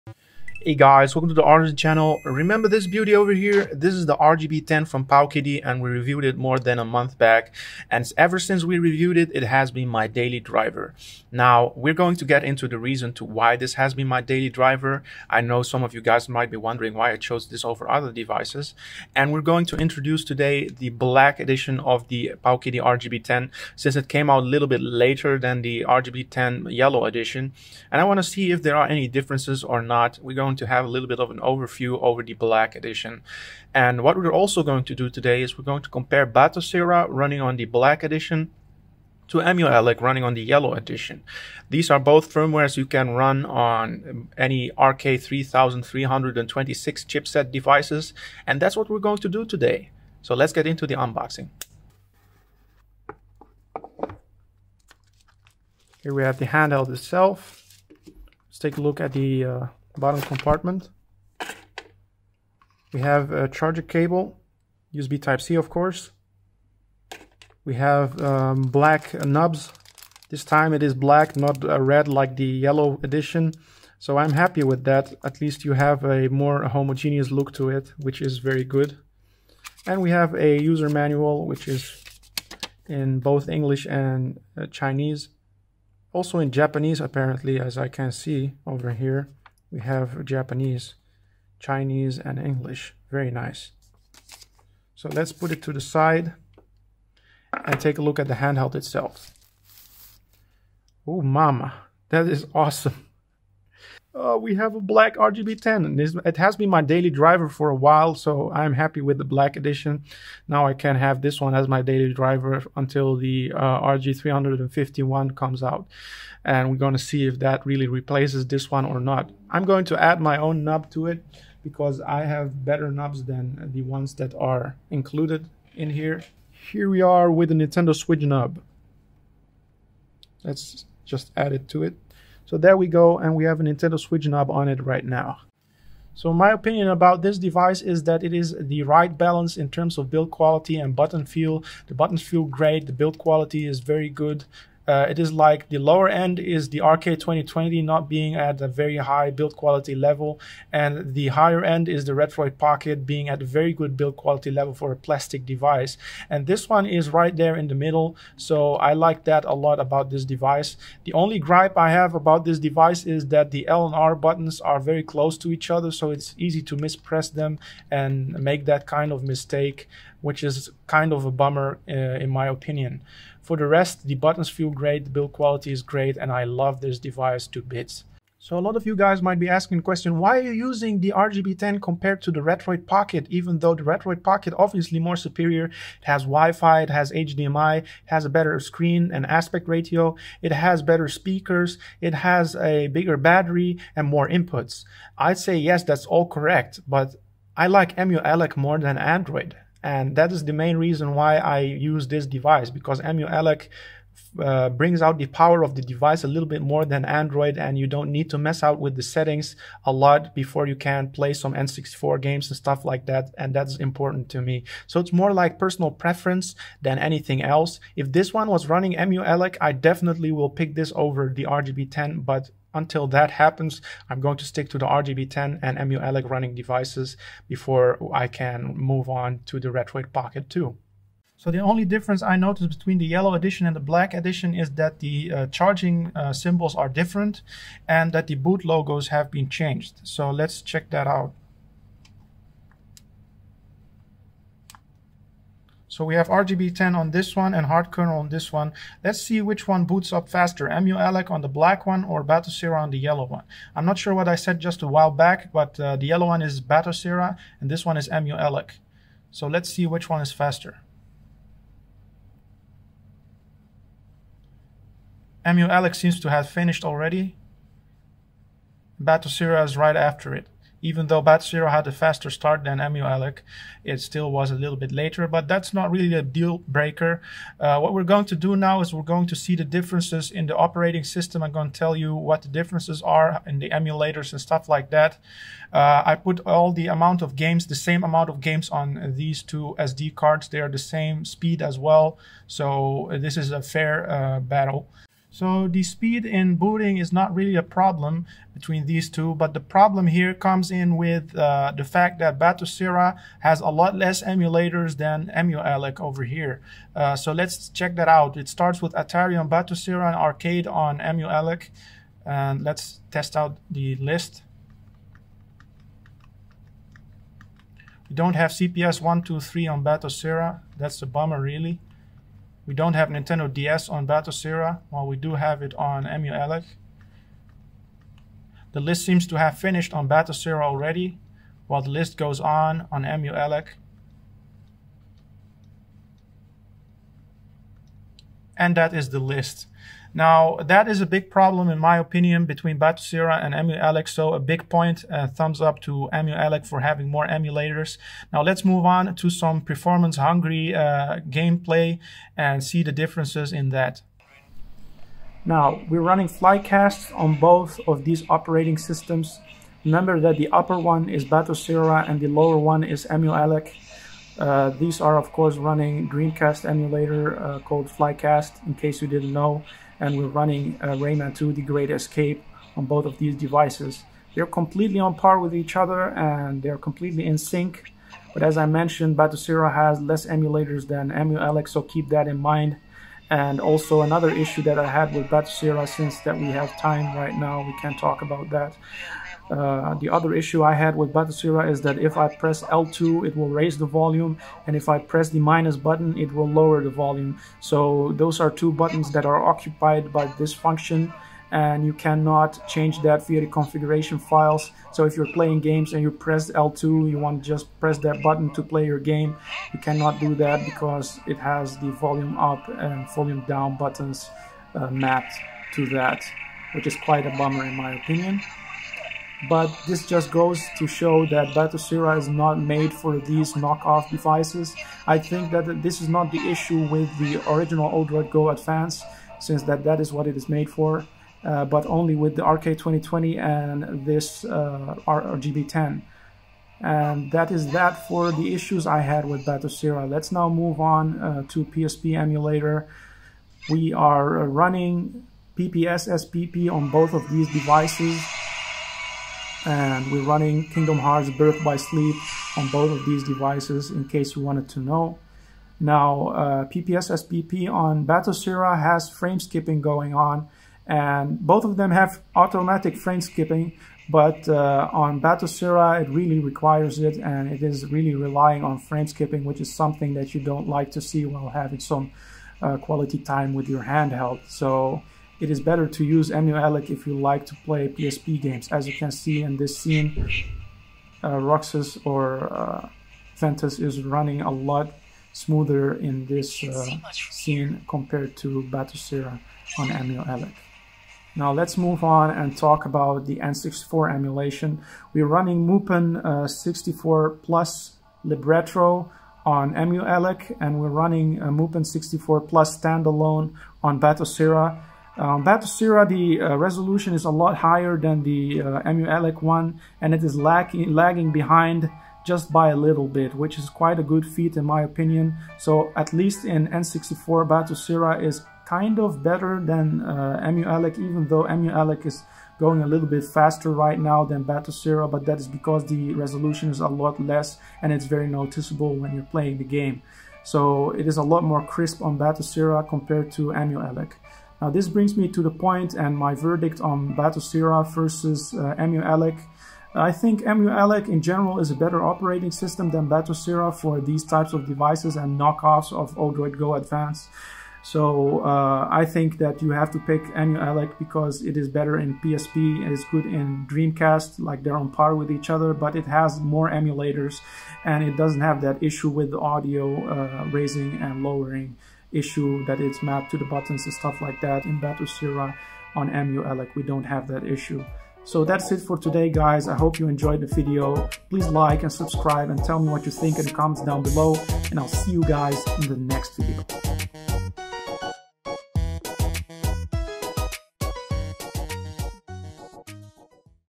RGB10 Black - Batocera vs Emuelec & Why I Love This Device!
In this video we take a look at the powkiddy RGB10 black edition, see the differences, talk about why this is my daily driver and compare batocera to emuelec.